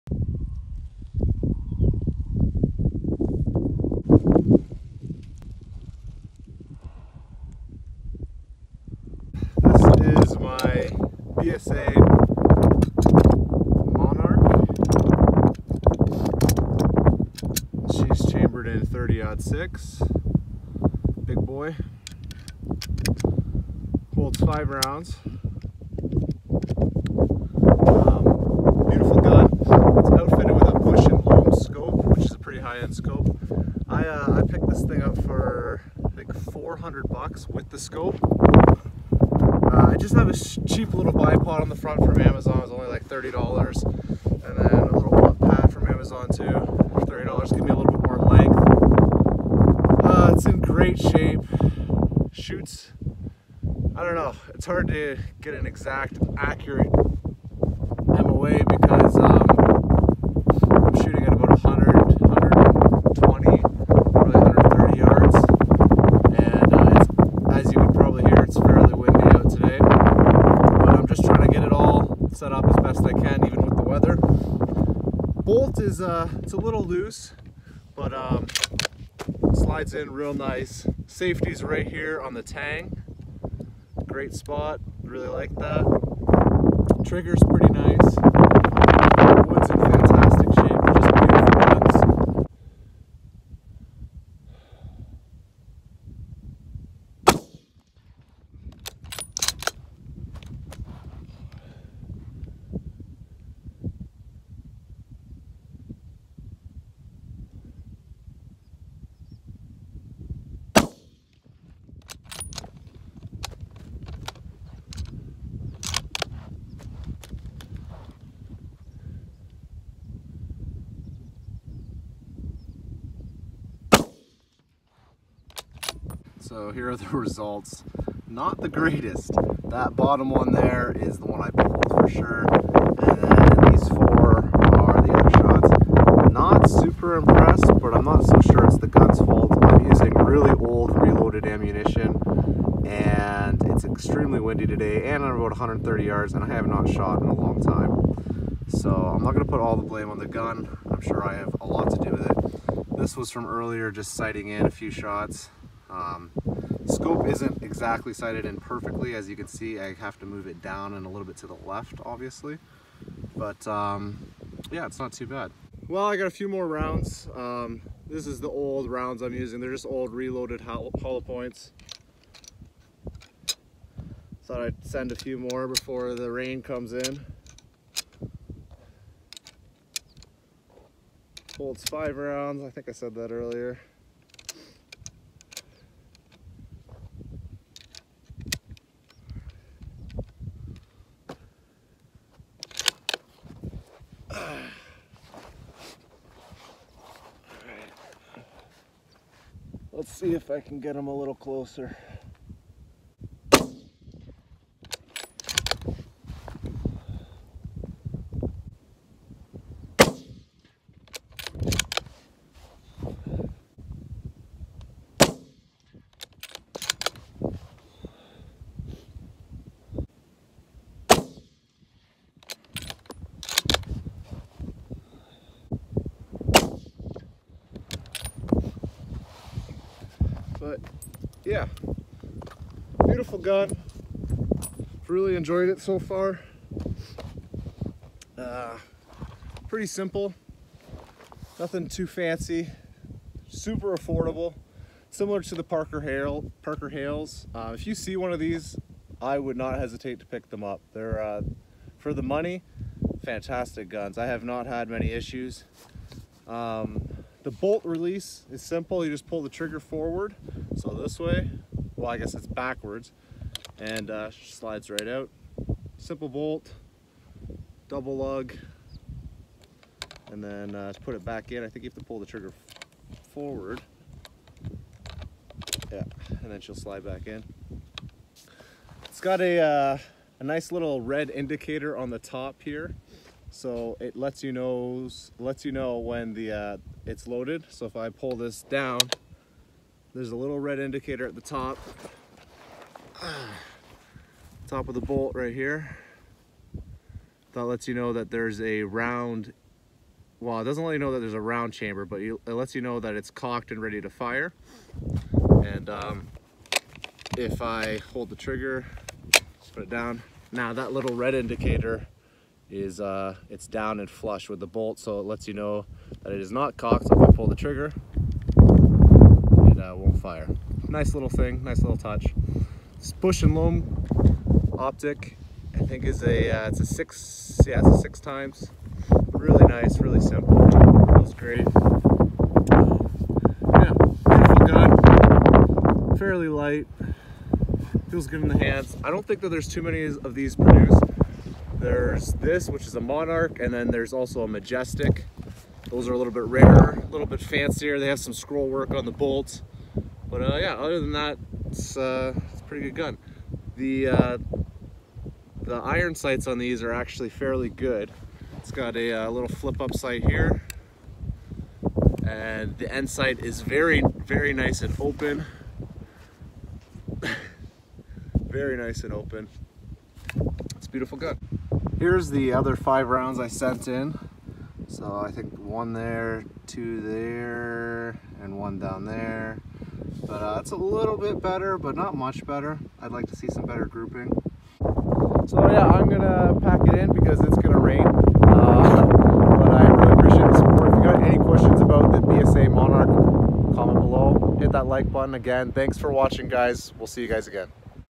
BSA Monarch Majestic chambered in 30-06
Trying out my BSA Monarch for some kind of long range shooting